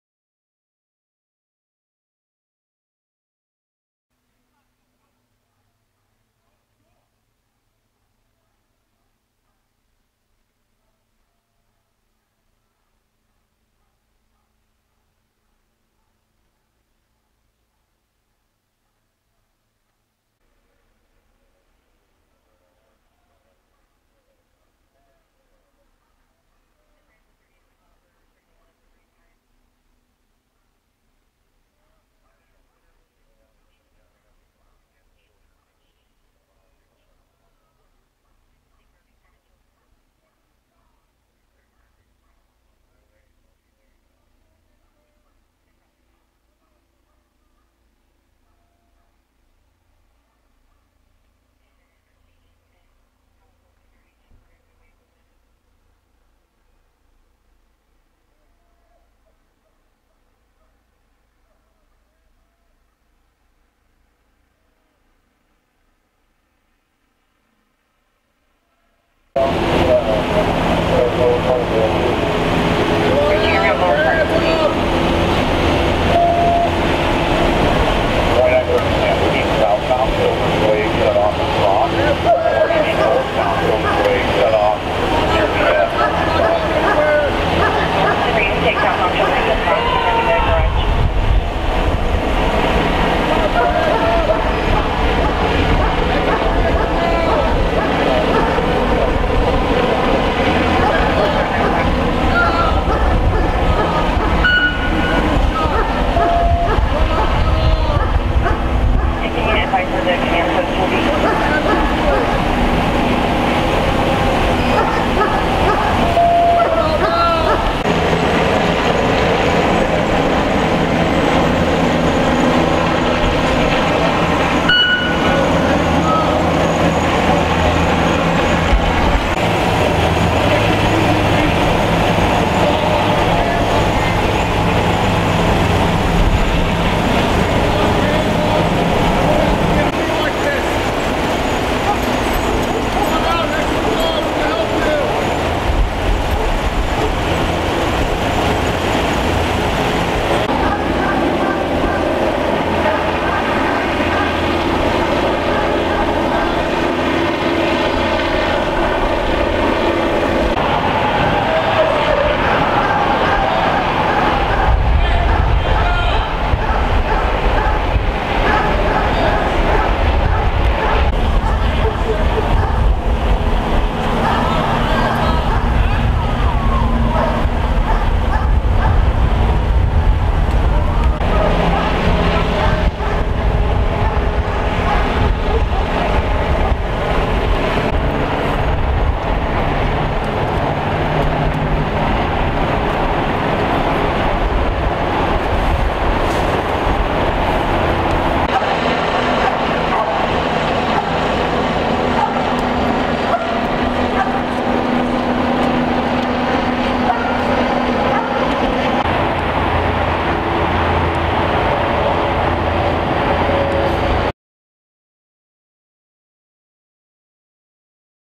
POLICE PURSUIT OF ARMED BANK ROBBER SUSPECT | RIVERSIDE CA
RIVERSIDE - On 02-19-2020, Riverside City Police Department responded to a report of an armed bank robbery that had occurred at a bank in the City of Riverside. The first arrived officer arrived on the scene and discovered a male adult entered the facility and demanded a large amount of cash with a gun and fled into a vehicle. The suspect was then located driving down Alessandro towards the 215 freeway. The suspect then entered the southbound 215 freeway when Riverside Police attempted to conduct a traffic stop but failed to yield and a pursuit ensued. The suspect led police from the City of Riverside and ended in the City of San Diego. The suspect stopped at a hospital and refused to surrender to the police. In the early morning hours, the suspect sustained a self-inflicted gunshot wound and was pronounced deceased. The investigated being investigated by Riverside City Police.